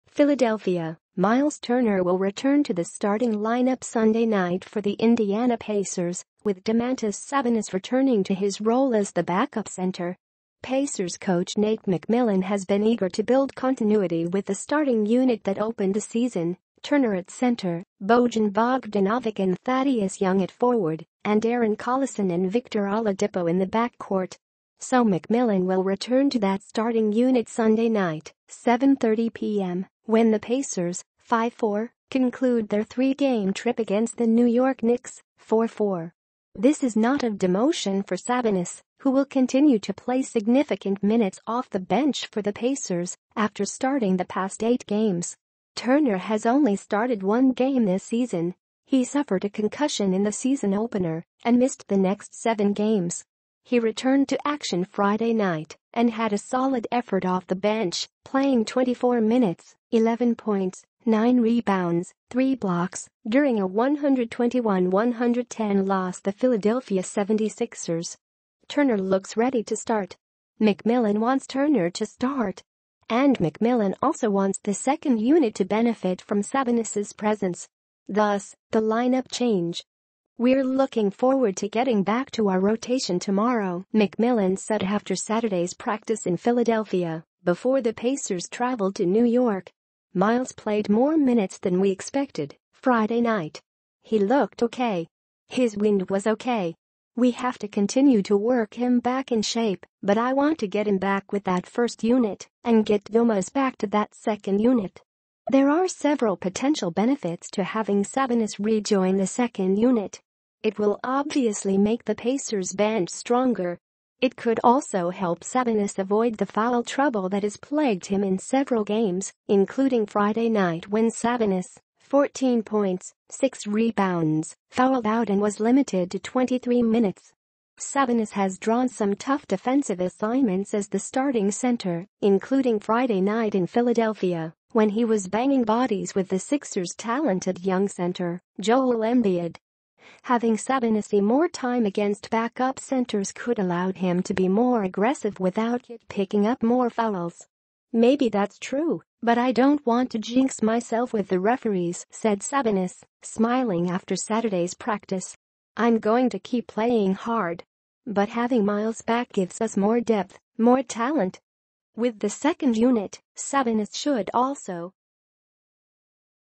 Myles Turner will return to Pacers starting lineup vs. Knicks on Sunday
PHILADELPHIA – Myles Turner will return to the starting lineup Sunday night  for the Indiana Pacers, with Domantas Sabonis returning to his role as the backup center.

Pacers coach Nate McMillan has been eager to build continuity with the starting unit that opened the season — Turner at center, Bojan Bogdanovic and Thaddeus Young at forward, and Darren Collison and Victor Oladipo in the backcourt. So McMillan will return to that starting unit Sunday night (7:30 p.m.), when the Pacers (5-4) conclude their three-game trip against the New York Knicks (4-4).

This is not a demotion for Sabonis, who will continue to play significant minutes off the bench for the Pacers, after starting the past eight games. Turner has only started one game this season; he suffered a concussion in the season opener and missed the next seven games. He returned to action Friday night and had a solid effort off the bench, playing 24 minutes (11 points, nine rebounds, three blocks) during a 121-110 loss the Philadelphia 76ers.

Turner looks ready to start. McMillan wants Turner to start. And McMillan also wants the second unit to benefit from Sabonis' presence. Thus, the lineup change.

"We're looking forward to getting back to our rotation tomorrow," McMillan said after Saturday's practice in Philadelphia, before the Pacers traveled to New York. "Myles played more minutes than we expected (Friday night). He looked okay. His wind was OK. We have to continue to work him back in shape, but I want to get